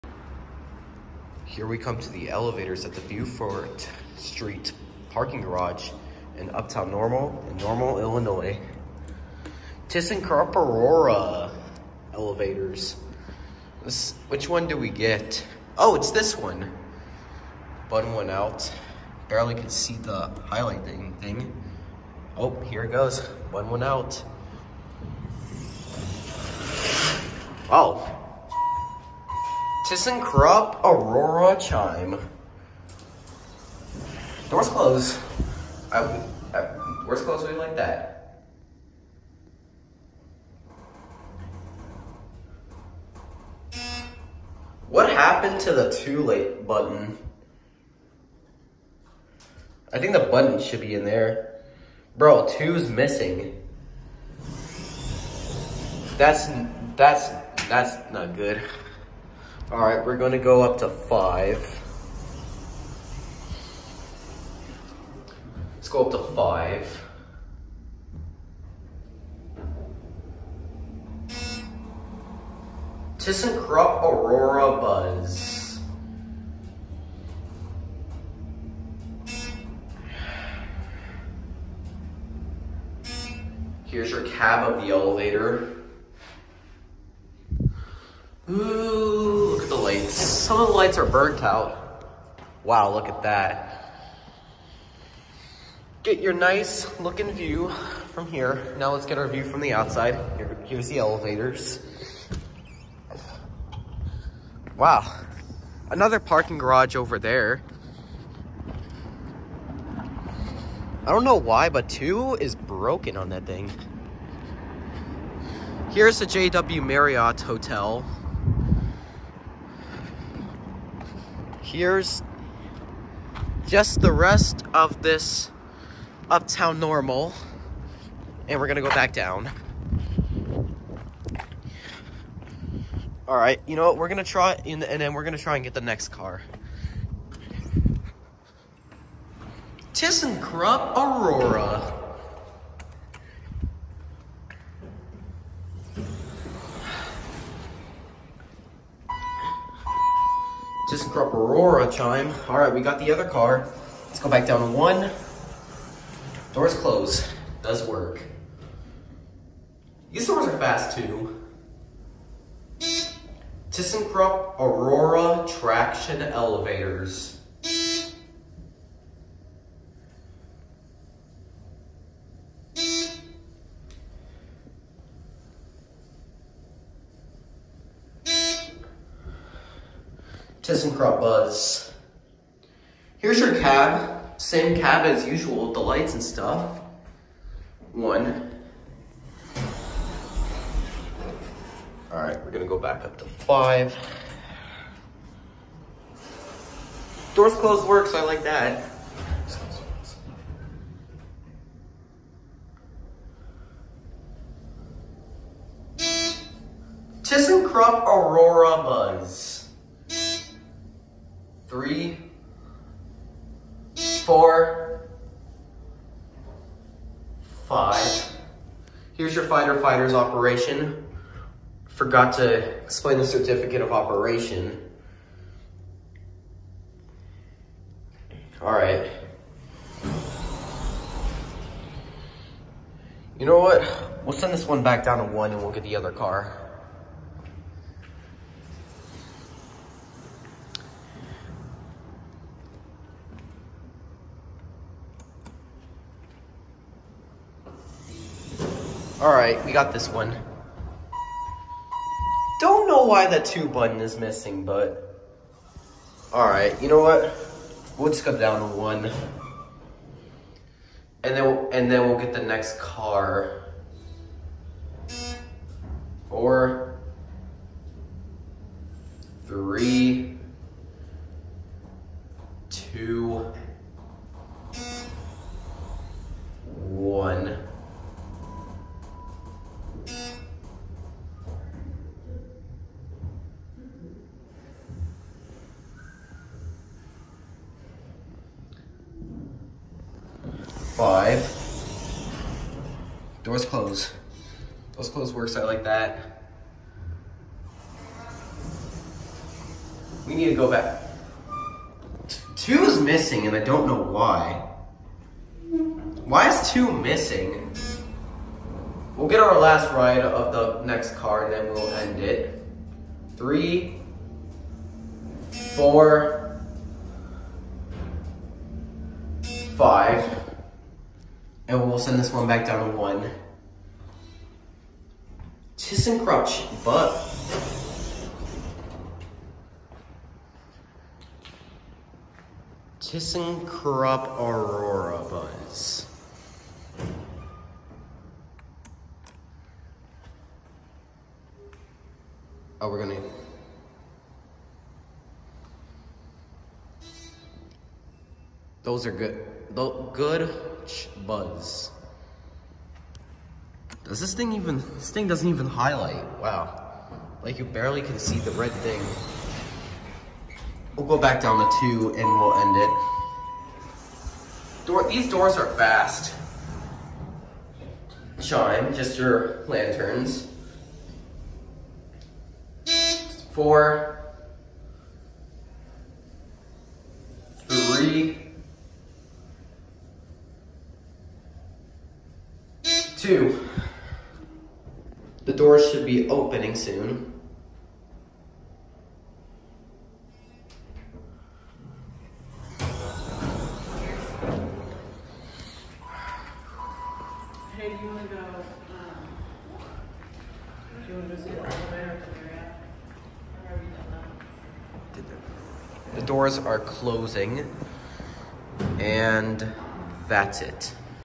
ThyssenKrupp Aurora Traction Elevators at Beaufort Street Parking Garage in Normal Illinois
This was also in Uptown Normal.